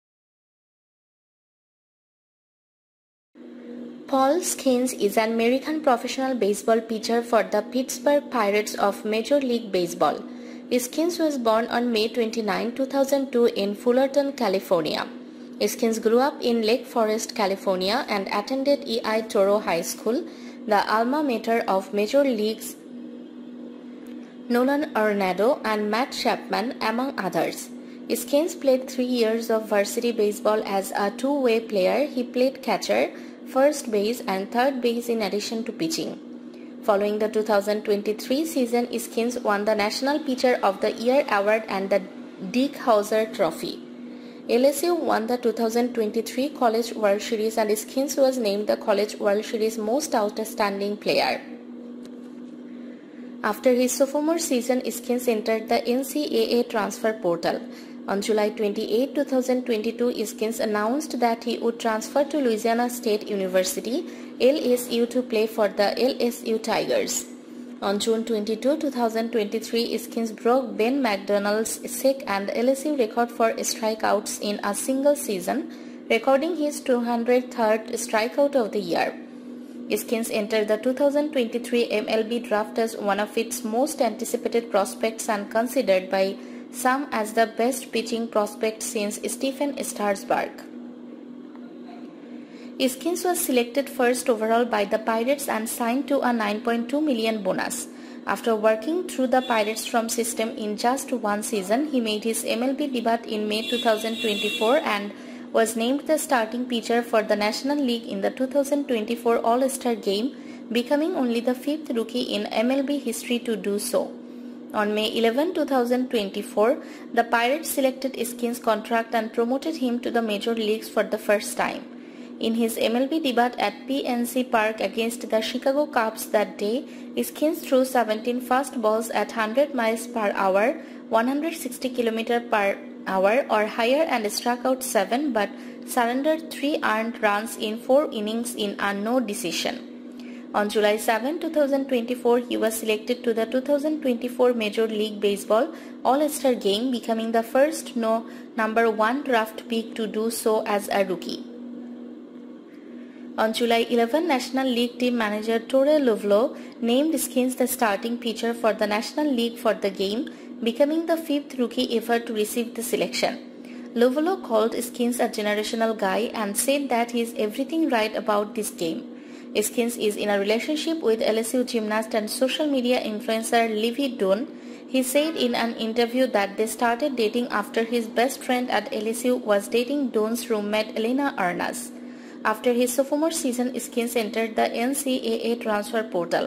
Paul Skenes || 7 Things You Need To Know About Paul Skenes
Paul Skenes is an American professional baseball pitcher for the Pittsburgh Pirates of Major League Baseball.
Skenes was born on May 29, 2002, in Fullerton, California.
Skenes grew up in Lake Forest, California, and attended El Toro High School, the alma mater of major leaguers Nolan Arenado and Matt Chapman, among others.
 Skenes played three years of varsity baseball as a two-way player — he played catcher, first base, and third base in addition to pitching.

Paul Skenes
Paul Skenes
Paul Skenes